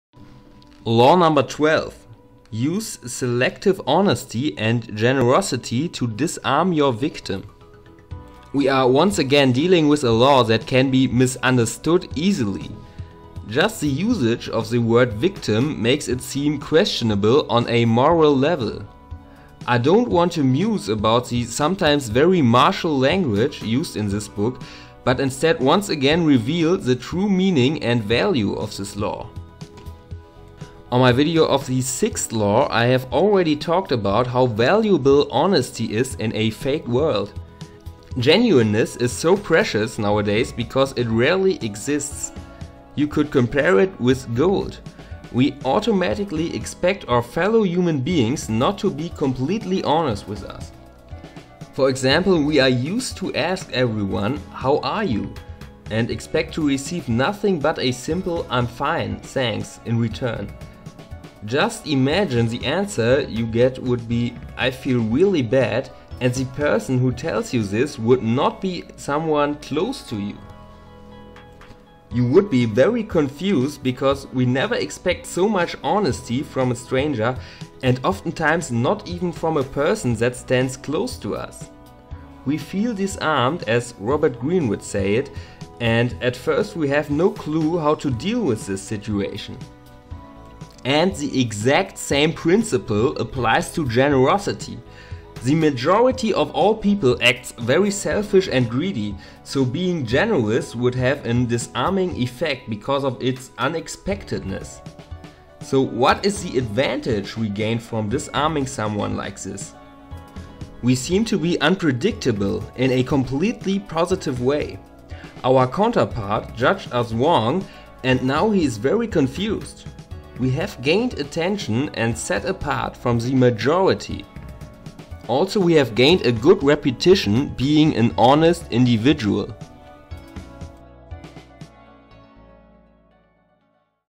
#12: Use Selective Honesty And Generosity To Disarm Your Victim | The 48 Laws Of Power
Law 12
Use Selective Honesty and Generosity to Disarm your Victim
One sincere and honest move will cover over dozens of dishonest ones. Open-hearted gestures
of honesty and generosity bring down the guard of even the most suspicious people. Once your
selective honesty opens a hole in their armor, you can deceive and manipulate them at will. A
timely gift – a Trojan horse – will serve the same purpose.

On Amazon.com
On Amazon.co.uk